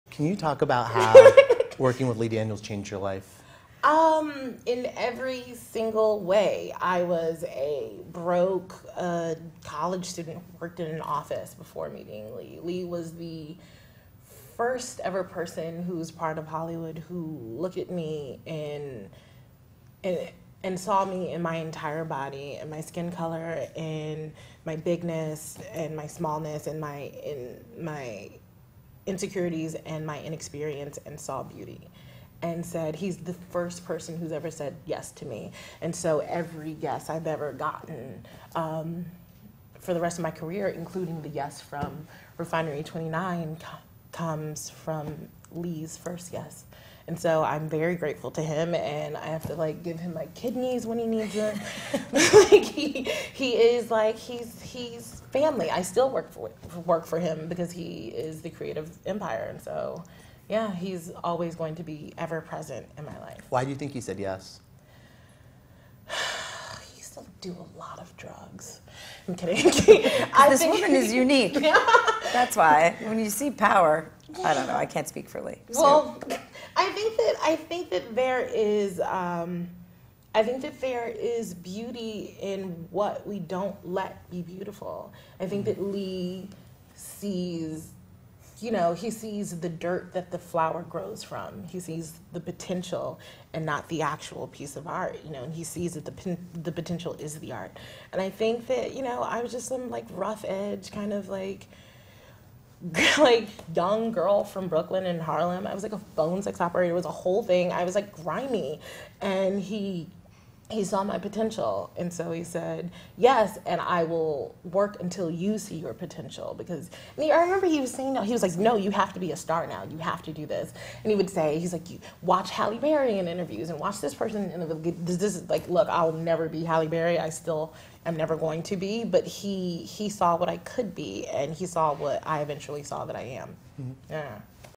Gabourey Sidibe on Lee Daniels: "I was grimy and he saw my potential"
Gabourey Sidibe talks to Variety about working with Lee Daniels in her breakout role, "Precious."